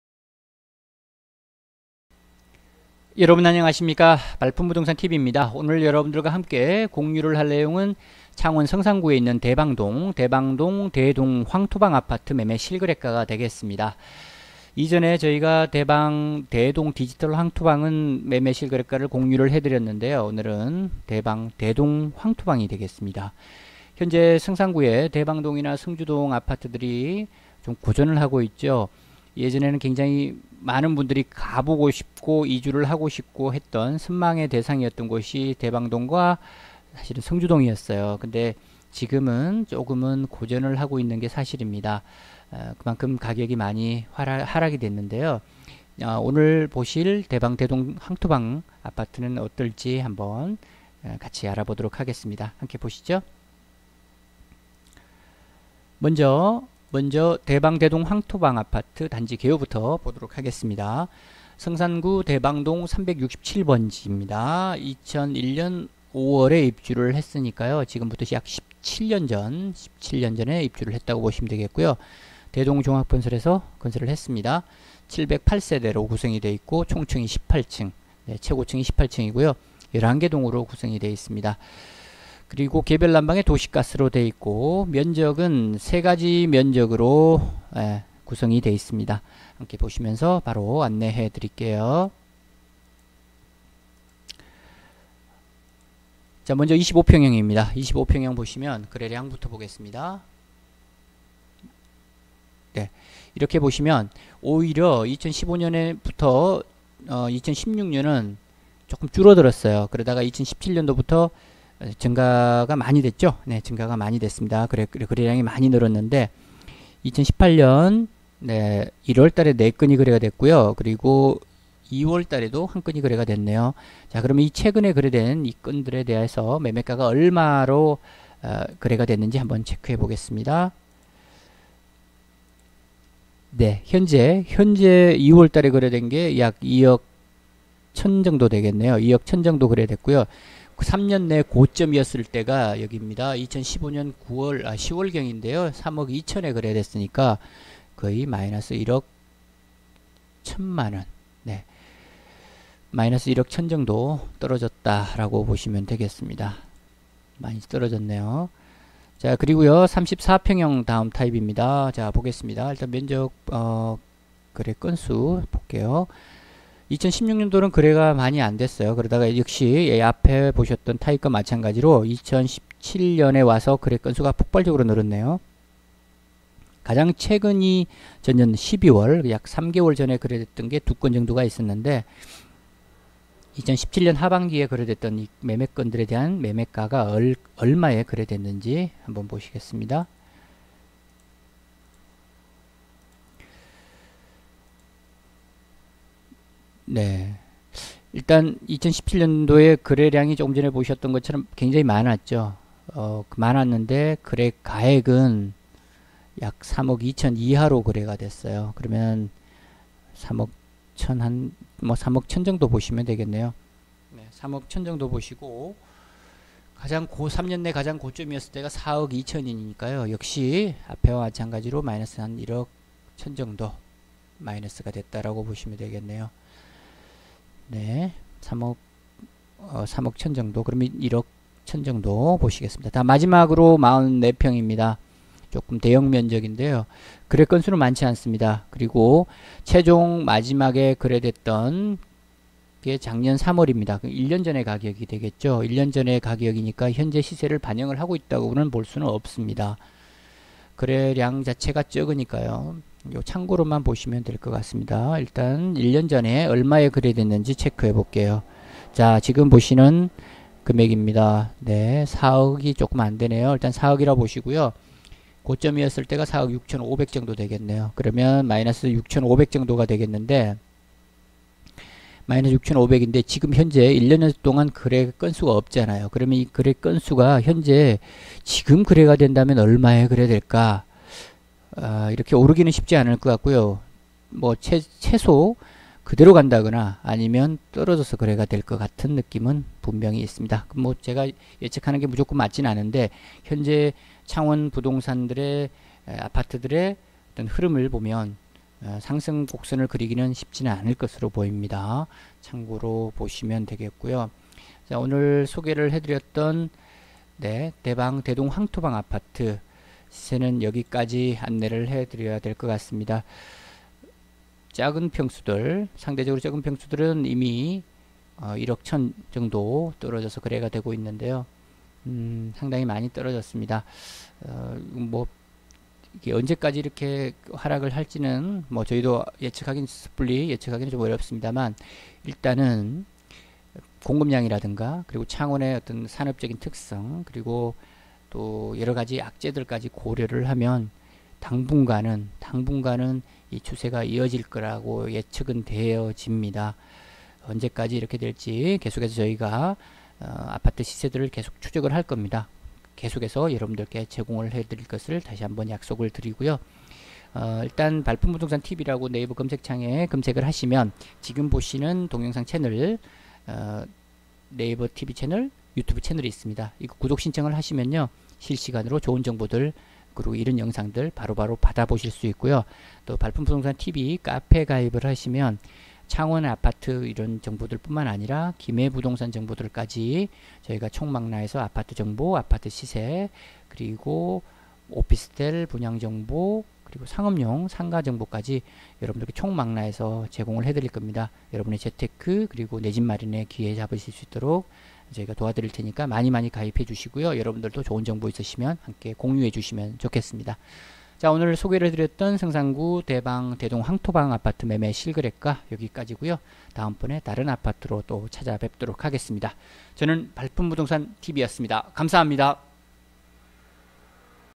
창원 아파트 시세 성산구 대방동 대동황토방 매매 실거래가 18년 3월 기준
창원 아파트 매매 시세 창원대동아파트 성산구 대방동 대동황토방 매매 실거래가 18년 3월 기준
발품부동산TV - 창원부동산 김해부동산 전문 카페